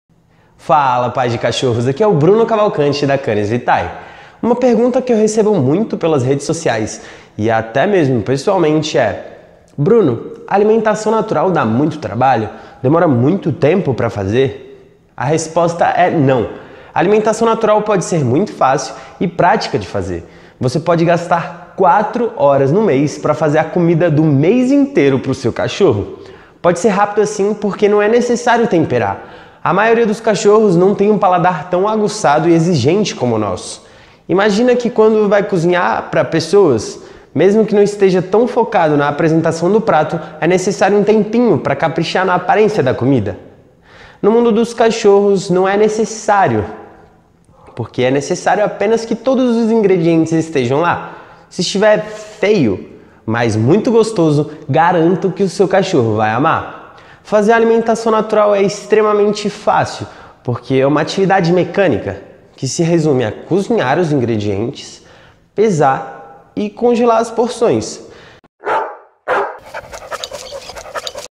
Demora Muito Tempo pra Fazer Alimentação Natural?
Bruno, Alimentação Natural dá muito trabalho, demora muito tempo pra fazer?
A resposta é não, Alimentação Natural pode ser muito fácil e prática de fazer.
Você pode gastar 4 horas pra fazer a comida do mês inteiro do seu cachorro.